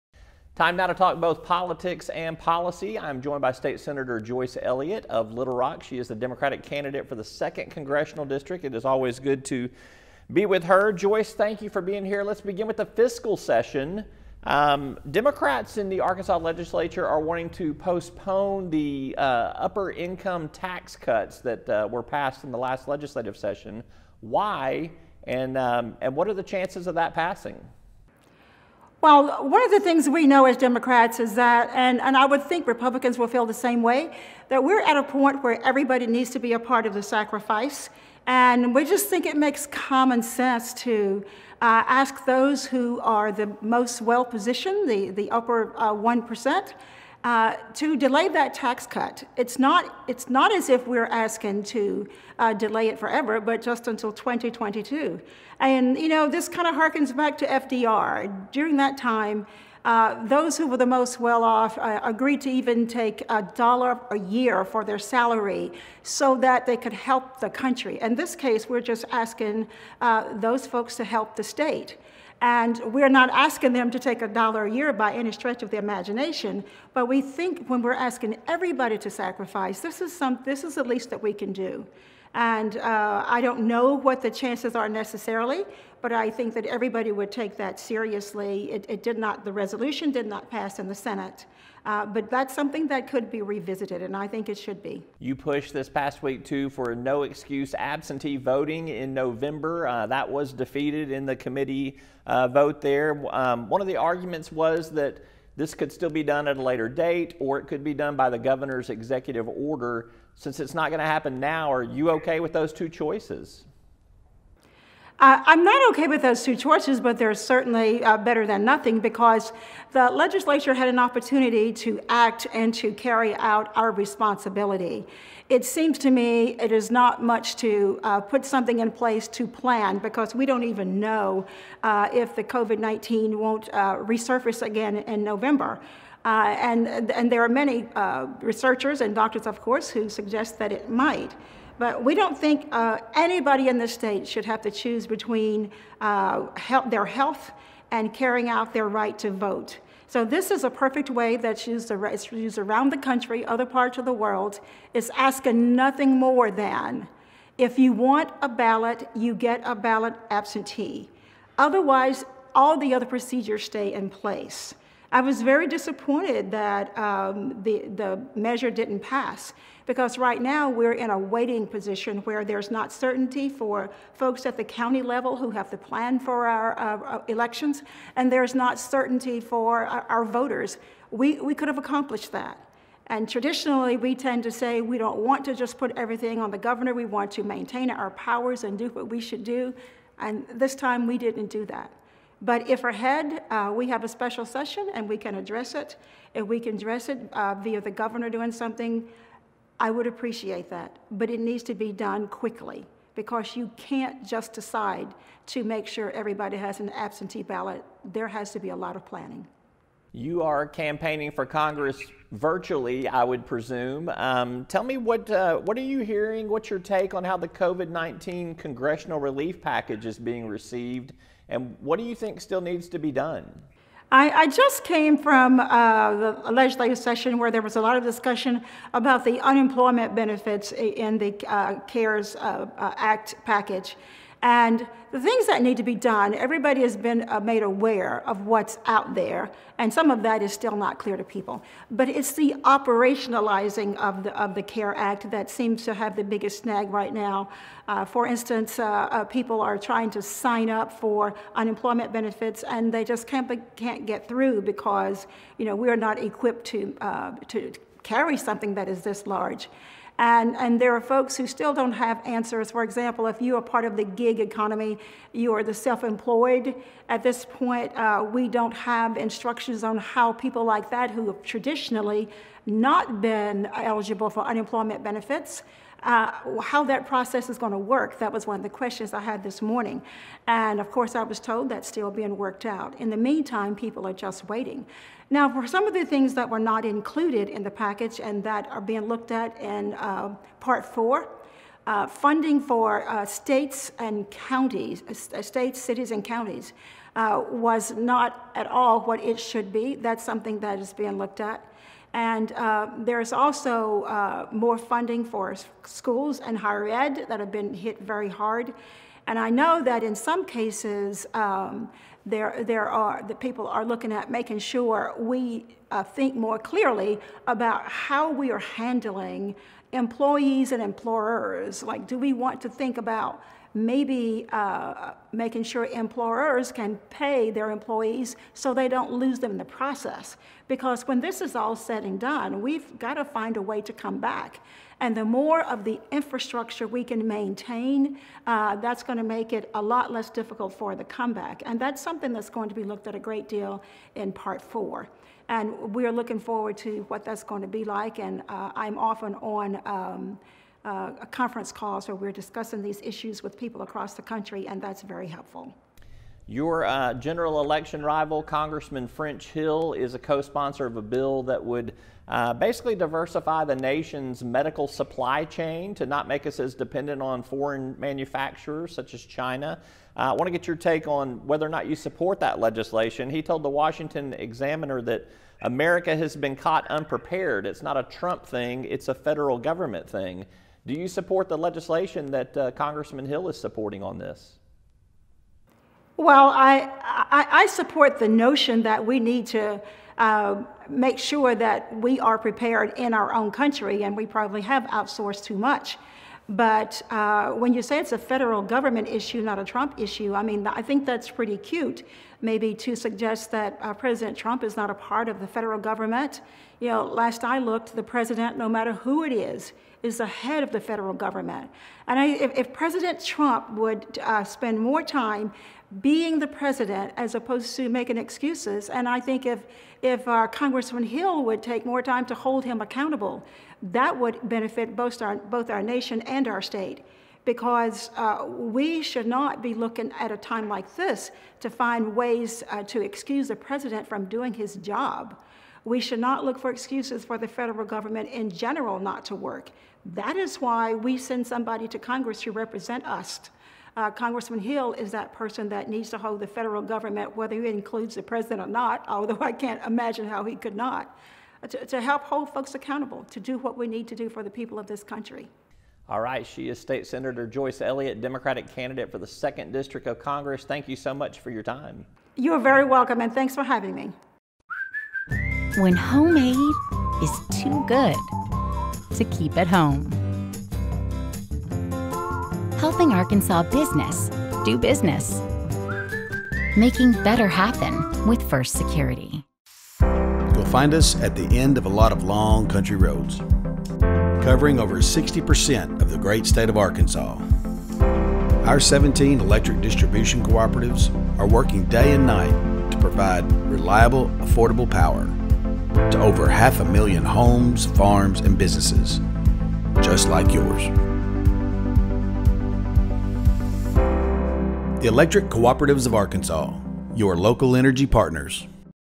Sen. Joyce Elliott 4.12.20
State Sen. Joyce Elliott (D-Little Rock), the Democratic candidate for 2nd District Congress, shares her perspective on what Congress and state lawmakers can do next to help those affected by the pandemic.